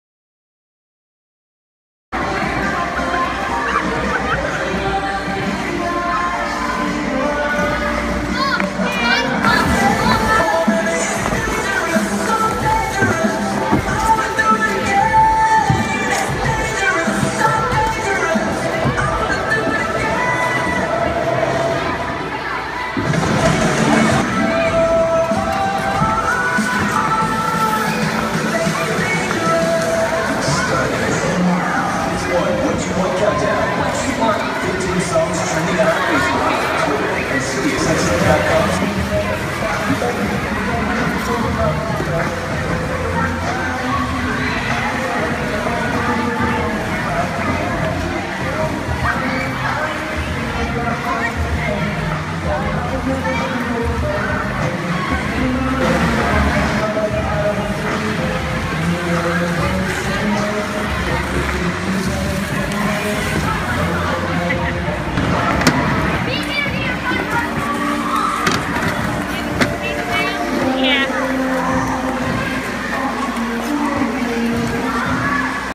Ice skating
Ice skating for Ella's 7th birthday